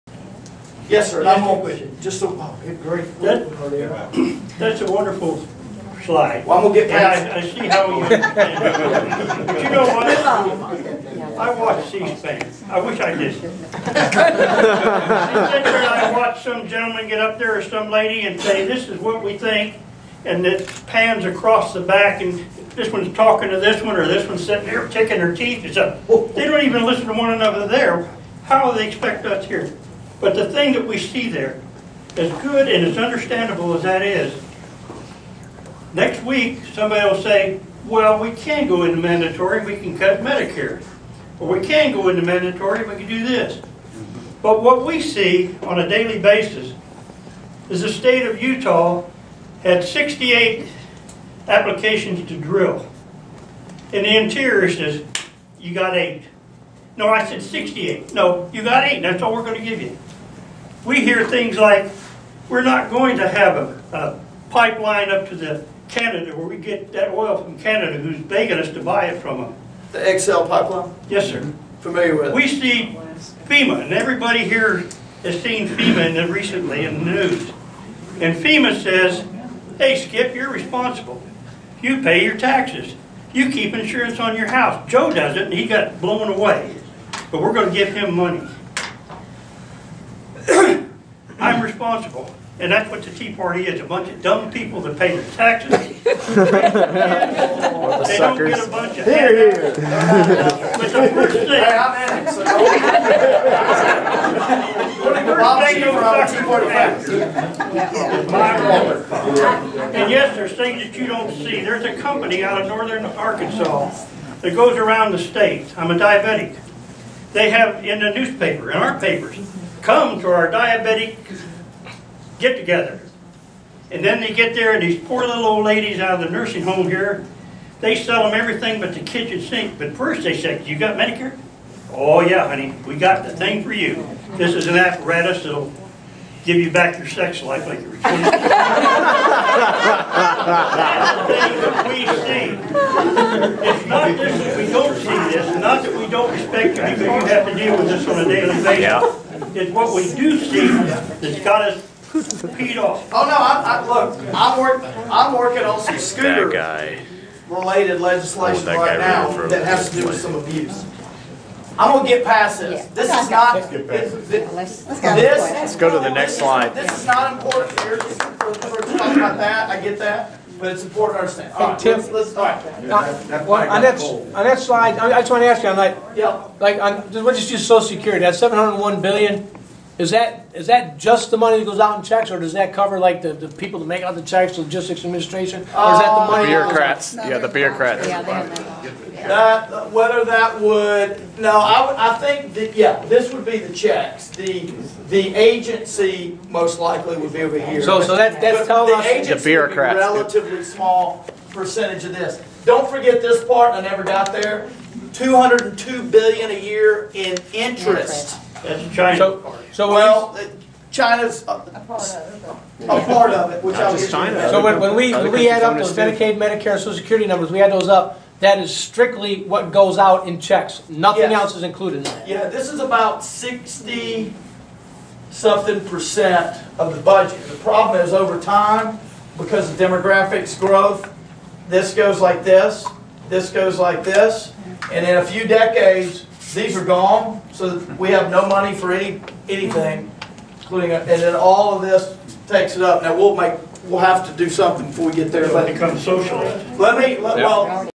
JAX Tea Party CSPAN and drilling for oil.wmv
The Jacksonville/Cabot Tea Party met on Tuesday May 16 2011. The guest speaker was United States congressman Tim Griffin of the 2nd congressional District of Arkansas. The Arkansas Tea Party attended the event and asked questions about spending and other issues. A Vietnam Veteran talks about the politics he sees on CSPAN and gives a specific example of how the federal government won't allow the states like Utah to drill for oil.